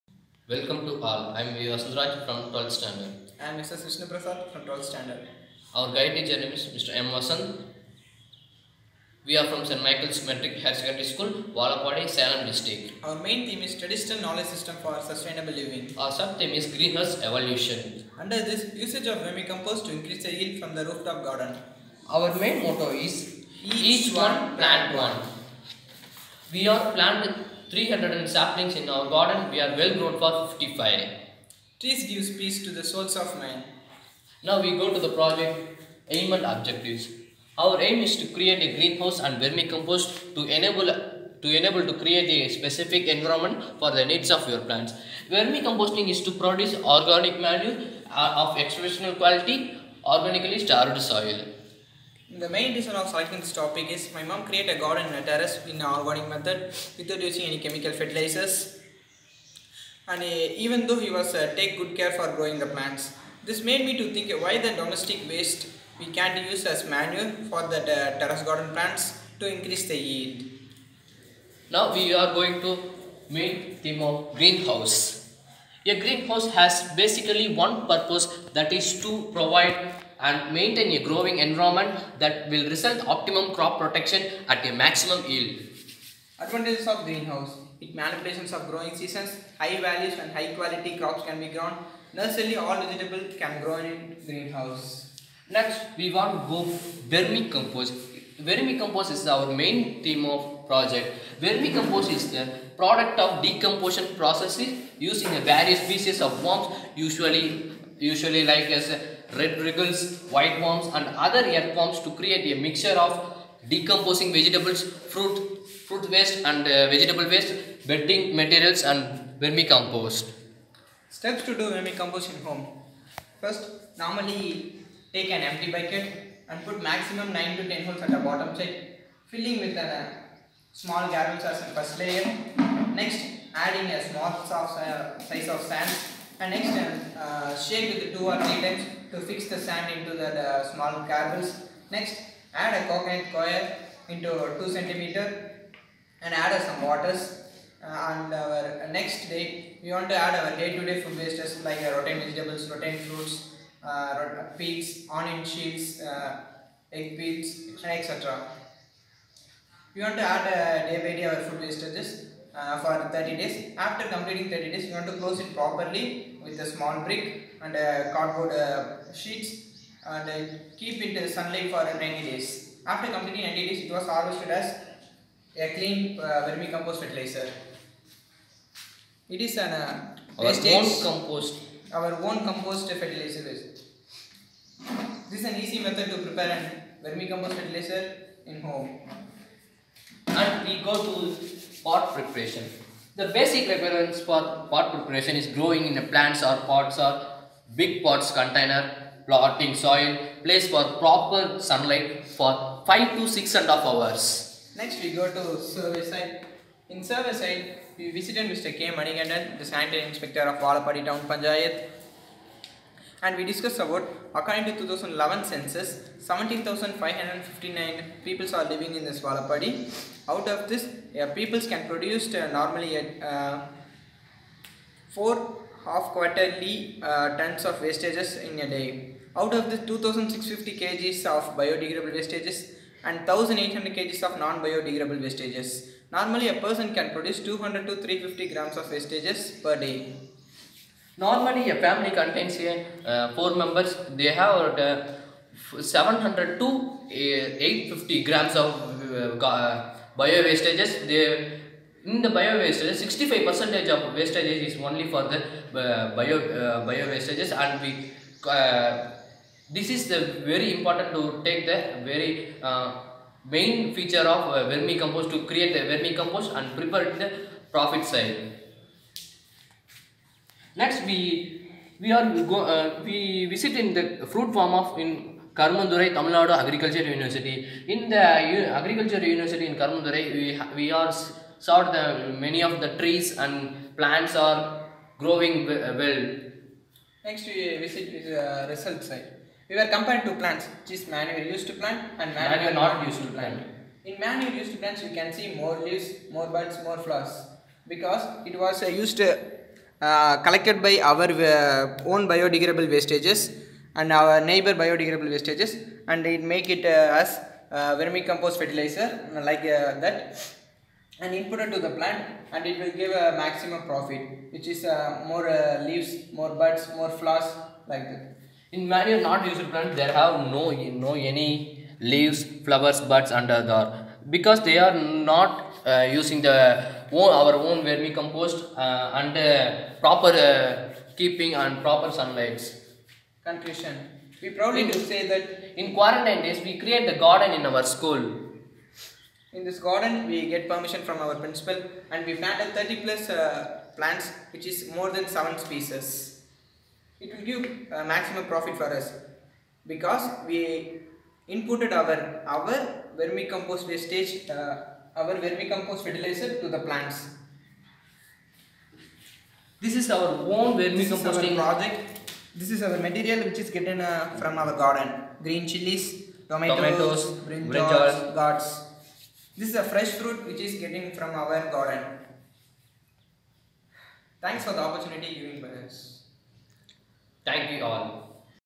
NSD2021: Exhibition - Senior Level - GreenHouse Evolution (Vermicompost)
NSD2021: Exhibition - Senior Level - GreenHouse Evolution (Vermicompost) - V. Vasantharaj & S. S. Vishnu Prasath

Name of Participant 1 : V.VASANTHARAJ
Class: XII -
School: ST.MICHAEL'S MATRIC HR SEC SCHOOL,TAMIL NADU,INDIA
Place:  VALAPADY,SALEM (DT), TAMIL NADU

Name of Participant 2 : S.S.VISHNU PRASATH
Class: XII -